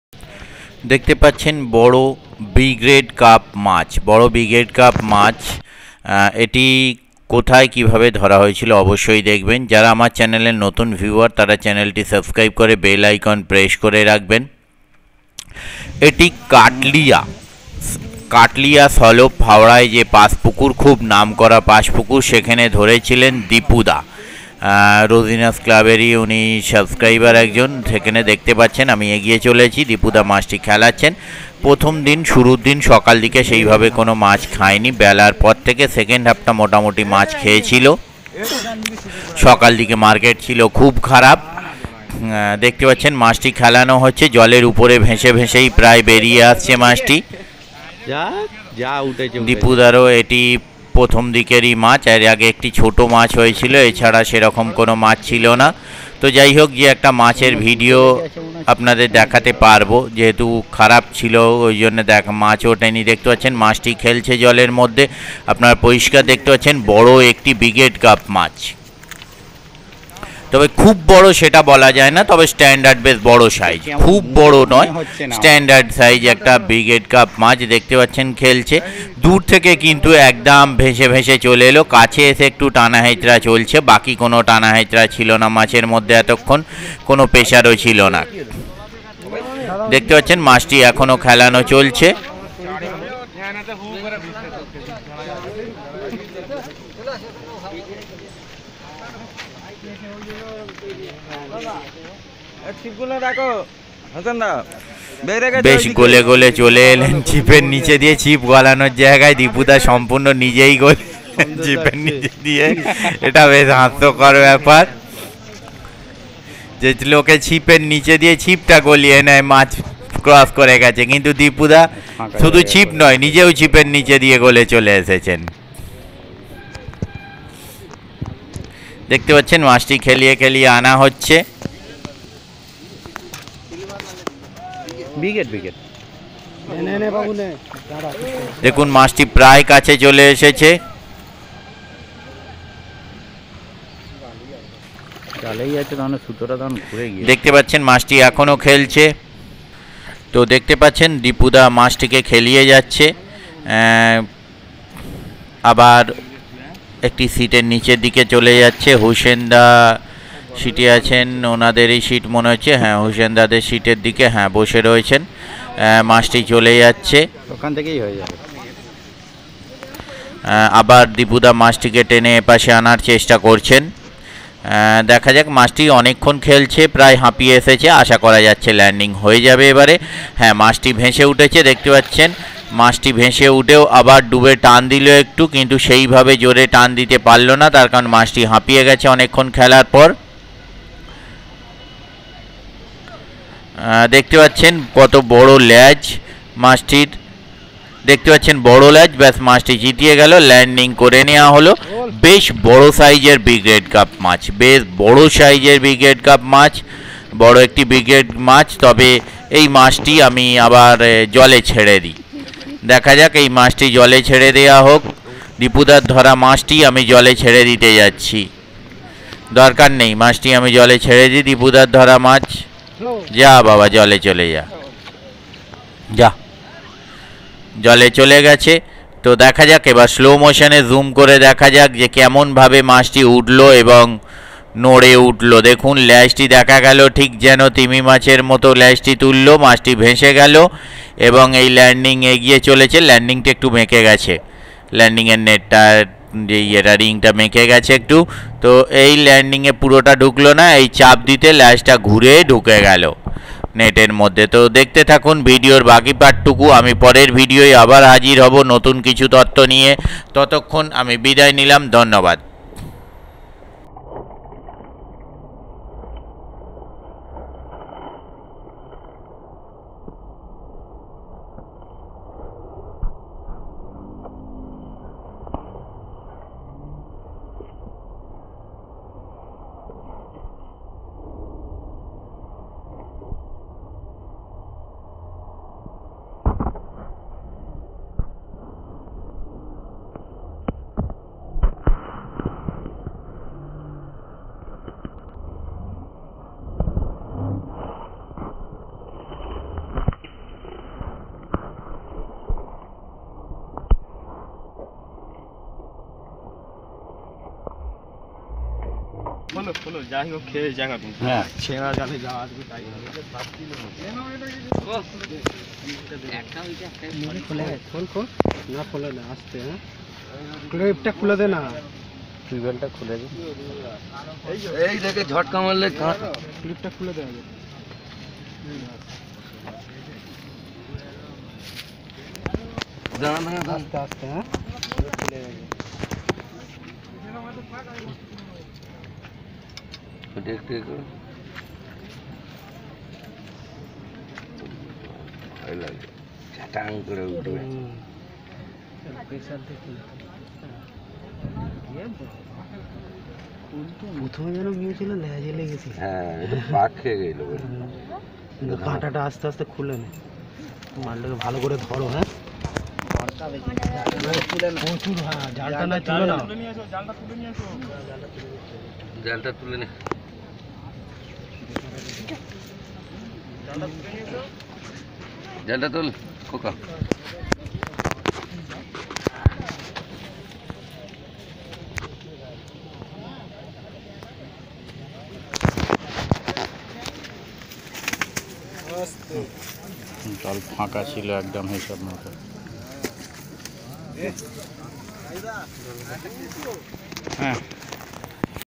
Big Briged Carp Fish at Katlia Pass Pond By Dipu Da | Rozina’s Club
The mrigal carp, also known as the white carp, is a species of ray-finned fish in the carp family. Native to streams and rivers in India, the only surviving wild population is in the Cauvery River, leading to its IUCN rating as vulnerable.